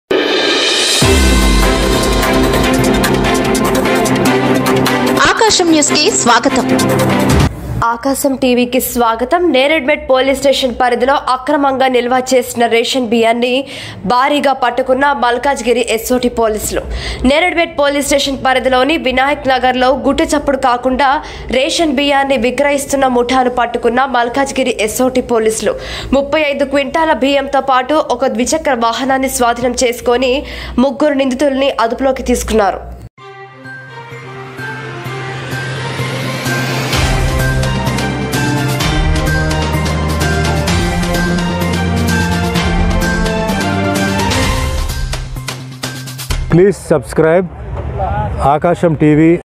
నెరేడ్ మెట్అక్రమంగా నిల్వ చేసిన రేషన్ బియ్యాన్ని భారీగా పట్టుకున్న మల్కాజిగిరిఎస్.ఓ.టి
నెరేడ్ మెట్ పోలీస్ స్టేషన్ పరిధిలో అక్రమంగా నిల్వ చేసిన రేషన్ బియ్యాన్ని భారీగా పట్టుకున్న మల్కాజిగిరి ఎస్.ఓ. టి పోలీసులు

   నెరేడ్మెట్ పోలీస్ స్టేషన్ పరిధిలోని వినాయక నగర్లో గుట్టుచప్పుడు కాకుండా రేషన్ బియ్యాన్ని విక్రయిస్తున్న ముఠా ను పట్టుకున్న మల్కాజిగిరి ఎస్.ఓ.టి పోలీసులు.
35 క్వింటాళ్ల బియ్యం తో పాటు ఒక ద్విచక్ర వాహనాన్ని స్వాధీనం చేసుకుని ముగ్గురు నిందితుల్ని అదుపులోకి తీసుకున్నారు.